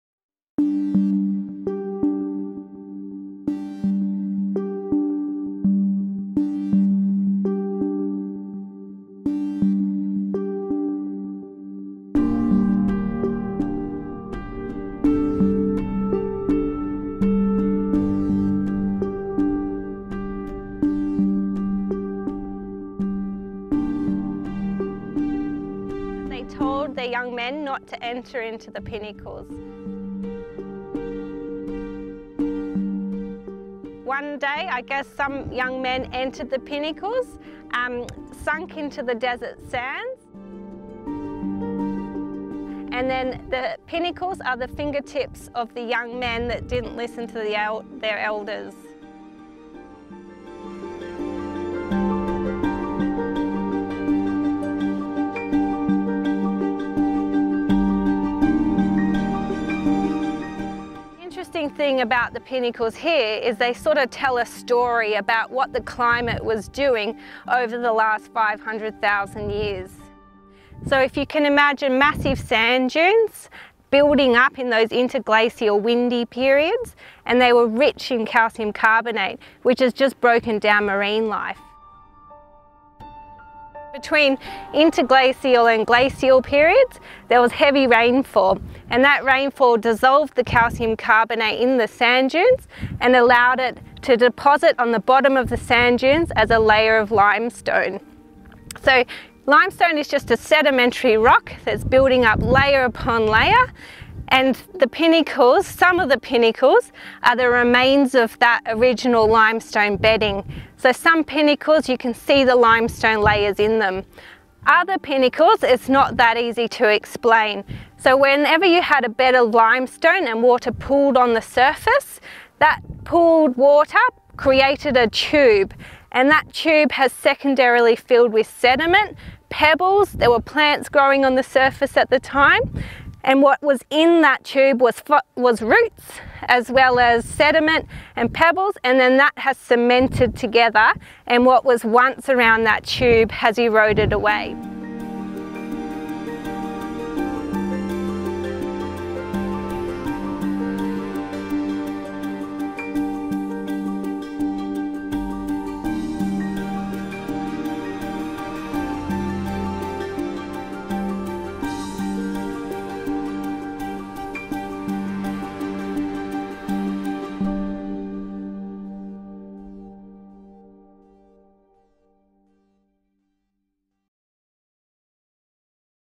How did The Pinnacles form? Indigenous and scientific explanations | ABC Australia
A few hours north of Perth, a cluster of ancient limestone rocks rise from the sand dunes as The Pinnacles Desert in Nambung National Park.

The Yued people say the pinnacles are the remains of young men lost to the shifting sands. But scientists have a far different explanation.

Take a journey through their mystical forms to discover the stories of their creation.